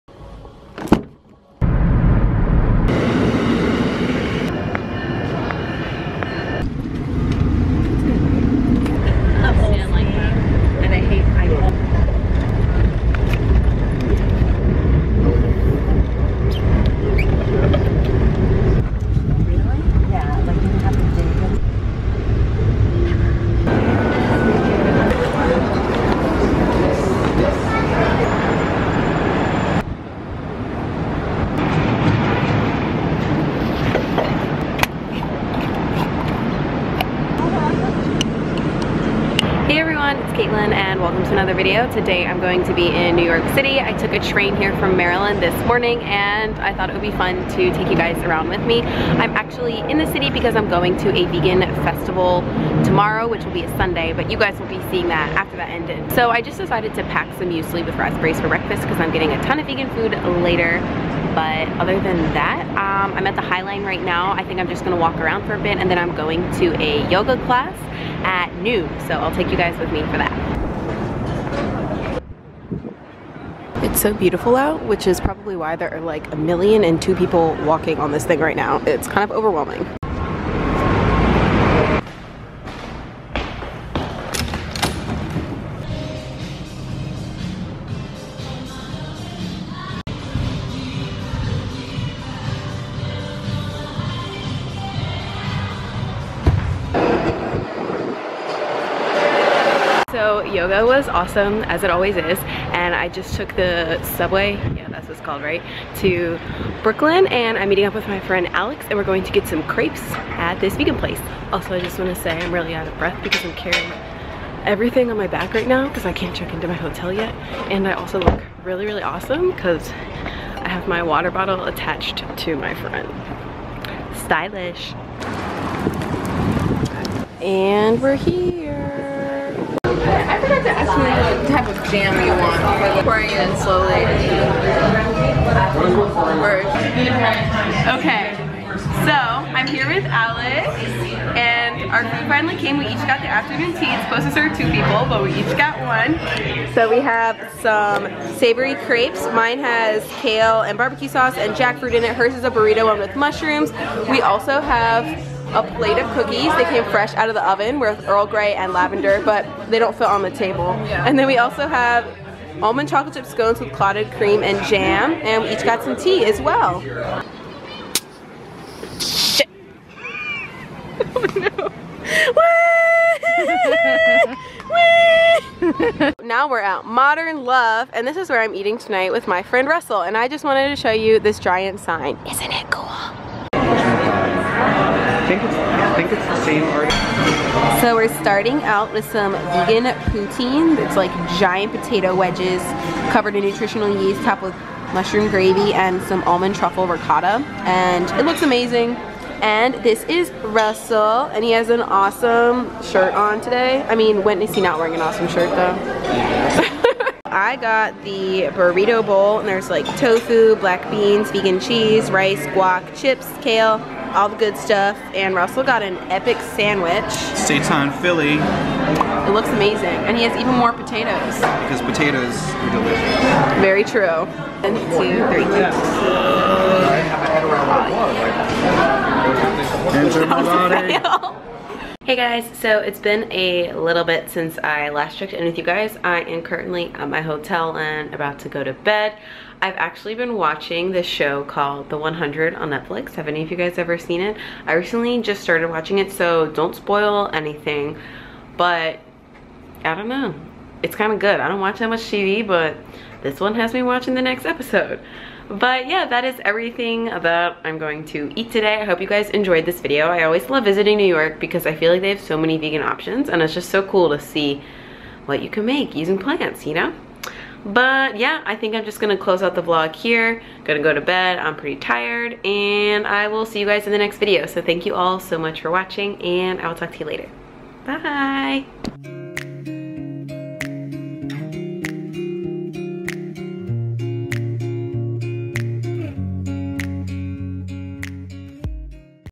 What I Ate + Did in NYC // Cozy Vegan Food 🍪
I spent this weekend in New York City, and here is some of the delicious Vegan Food that I ate!

• I walked at The High Line and took a yoga class at Laughing Lotus
• We ate at Little Choc Apothecary and Modern Love
• Shirt is from Spiritual Gangster!

• Cameras:
For Vlogging
For Photography + Recipes